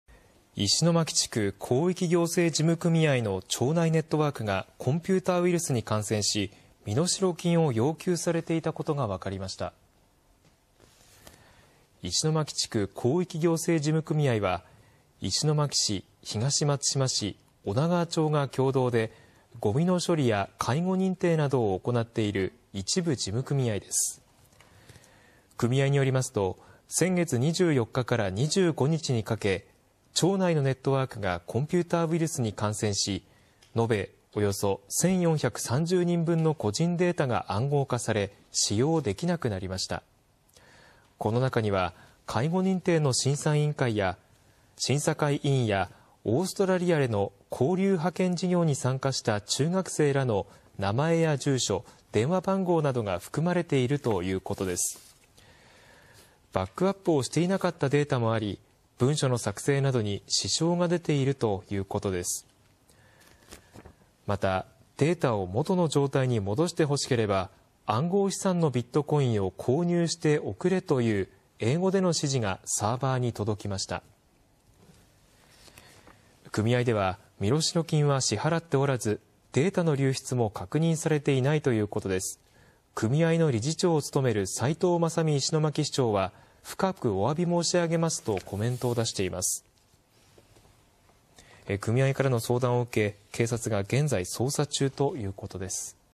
宮城・石巻地区の行政ネットワークがウイルスに感染し身代金を要求される
宮城県の石巻地区広域行政事務組合の庁内ネットワークがコンピューターウイルスに感染し、身代金を要求されていたことが分かりました。